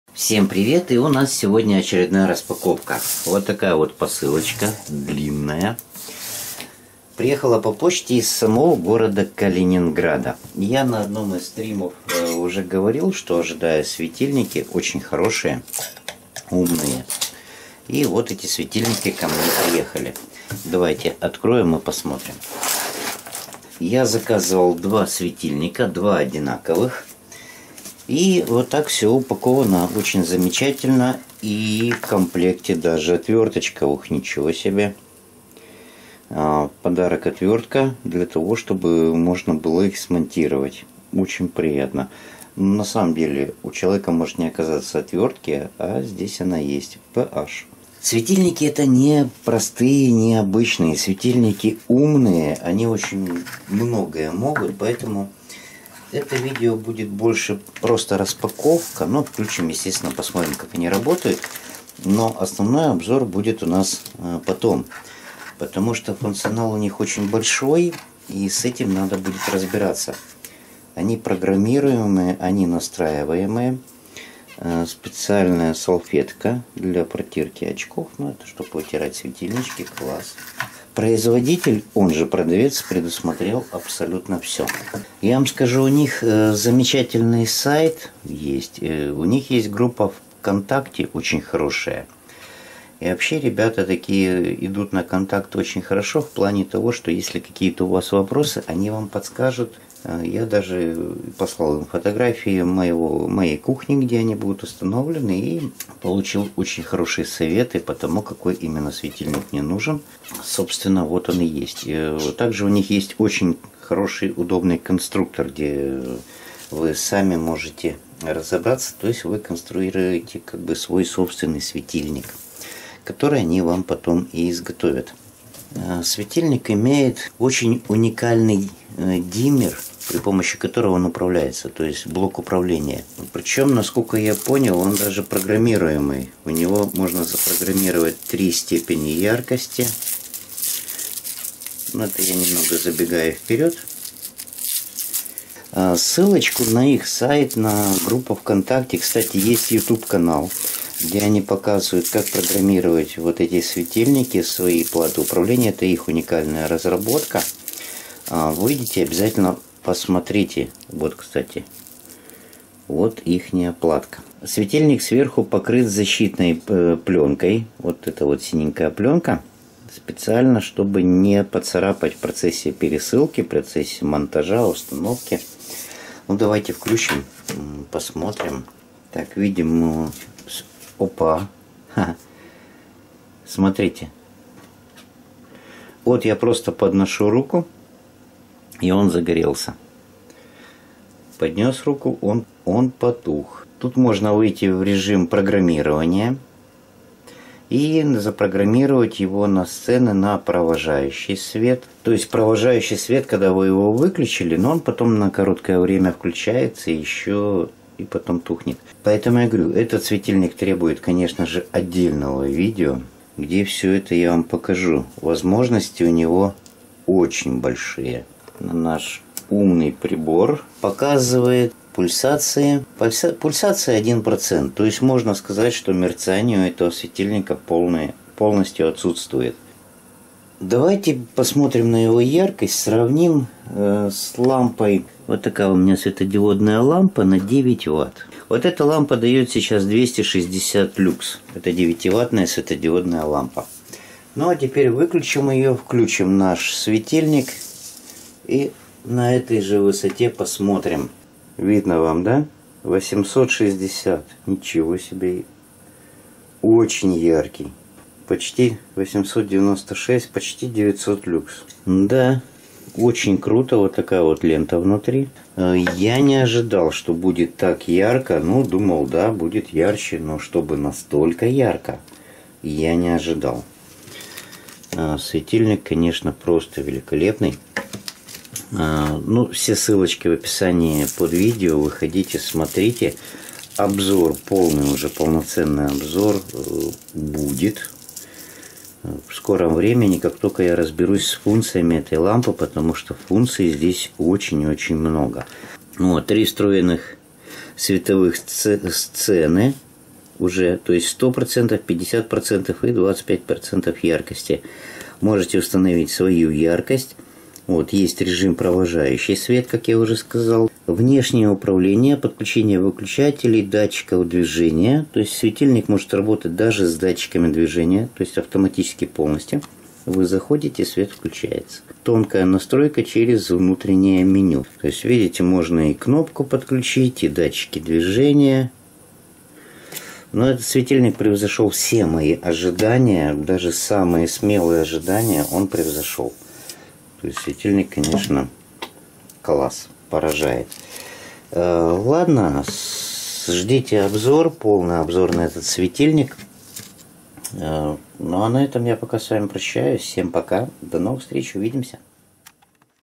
Светильники Fulogy со встроенным smart dimmer
Светодиодный светильник с бесконтактным сенсорным управлением Fulogy Smart-Dimmer для моей кухни.